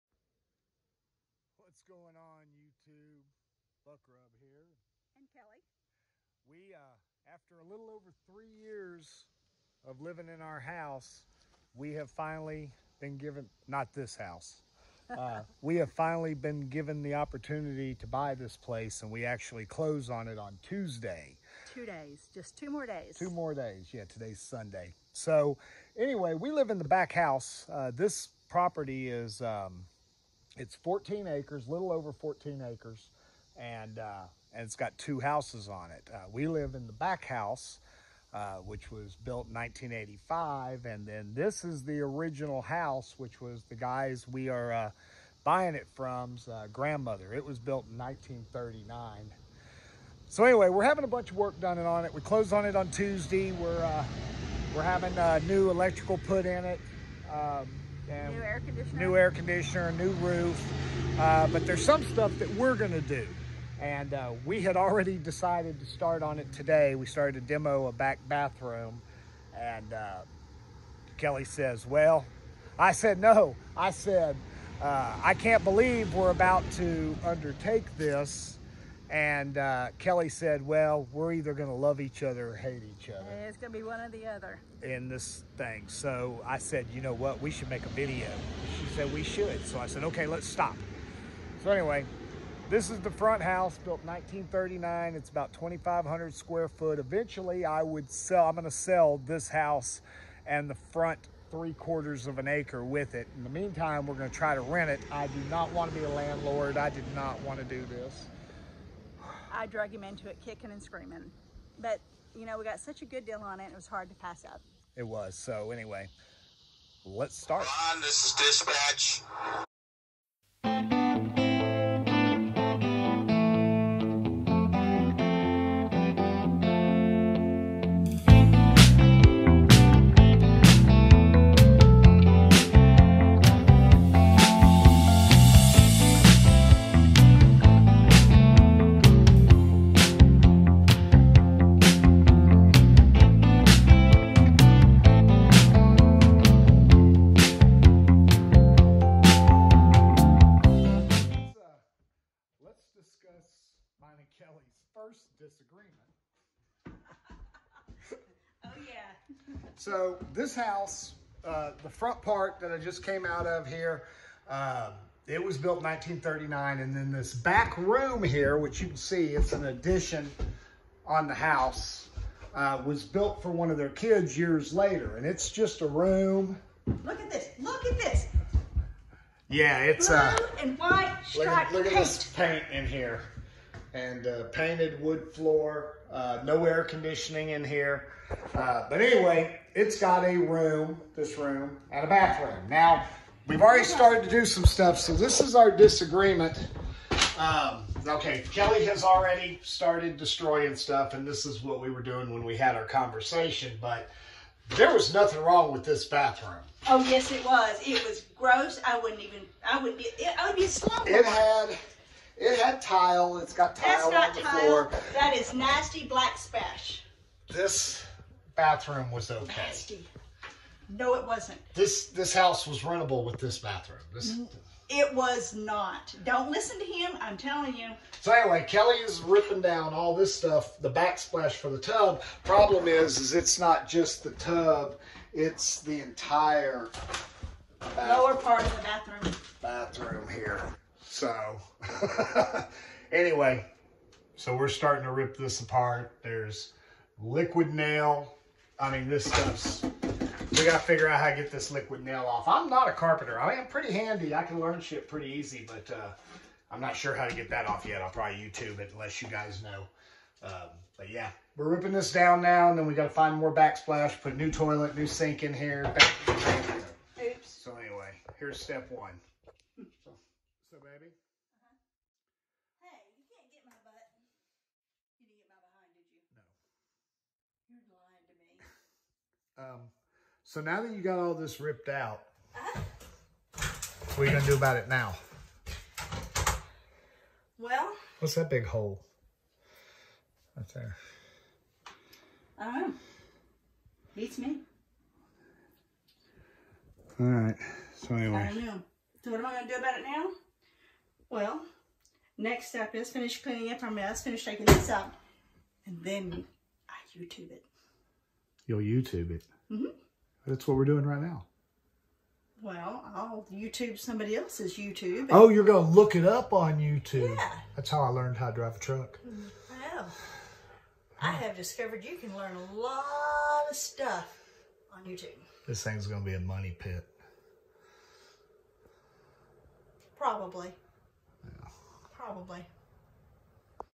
OperationRenovation part 1￼
Sorry about the volume at the beginning it fixes itself I’ll be more careful next time. Kelli and I have purchased two houses in some land the front house needs a lot of work we are going to try to chronicle what we do to it.